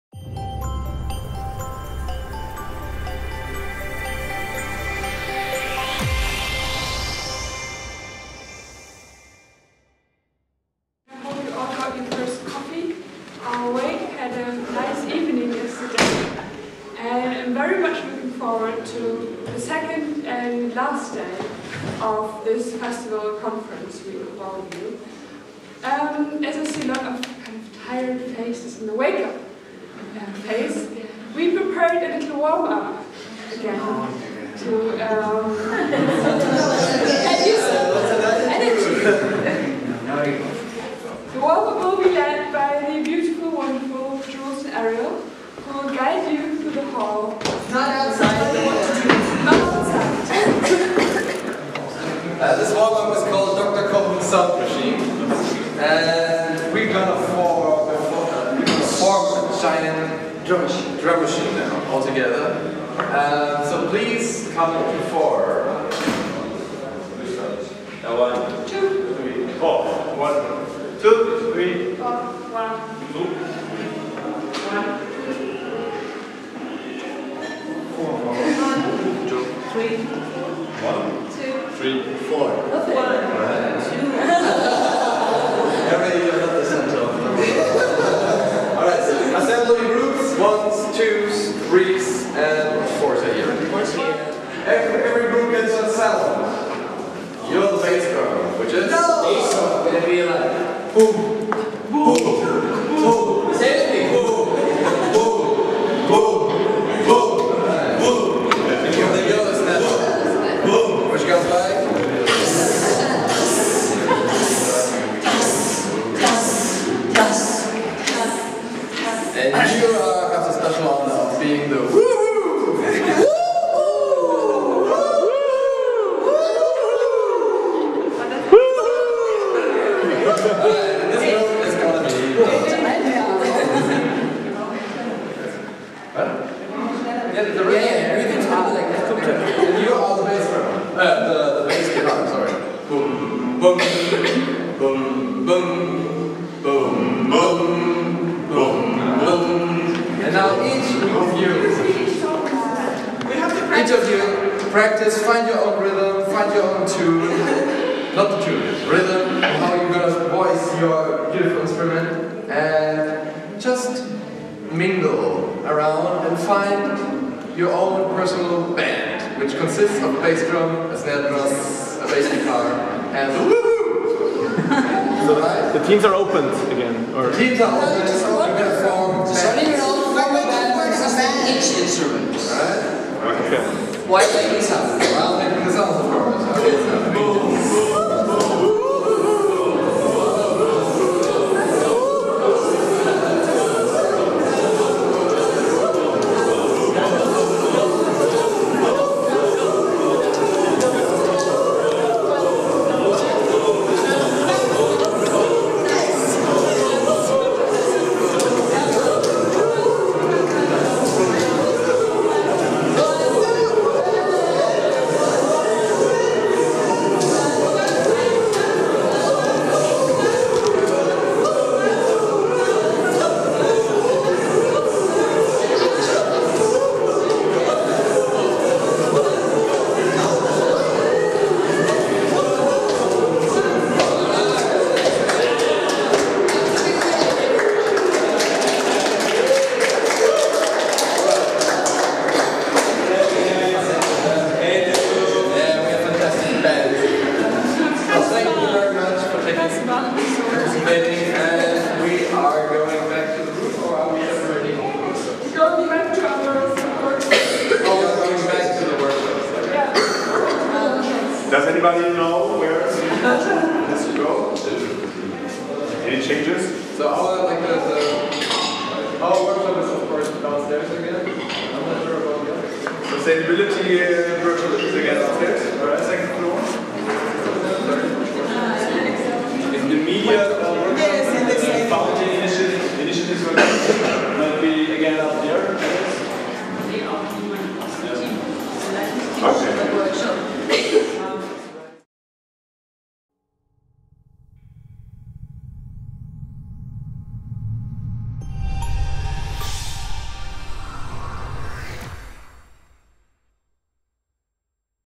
Check-In Runde zum Vernetzungstreffen studentischer Initiativen
Nach der Party am Vorabend aktivierten die Moderatorinnen und Moderatoren vom Design Thinking Collective der Luftmenschen zunächst alle Teilnehmenden durch einige "Aufwärmübungen". Anschließend wurde die am Vortag begonnene Arbeit fortgesetzt.

Das Video des Vernetzungstreffens studentischer Initiativen ist Teil der Veranstaltung "Engagierte Hochschule"

Videoproduktion:
Aufzeichnung: Radio Pegasus (Katholische Universität Eichstätt-Ingolstadt)
Nachbearbeitung: Jonathan Huber (Medienlabor der Universität Augsburg)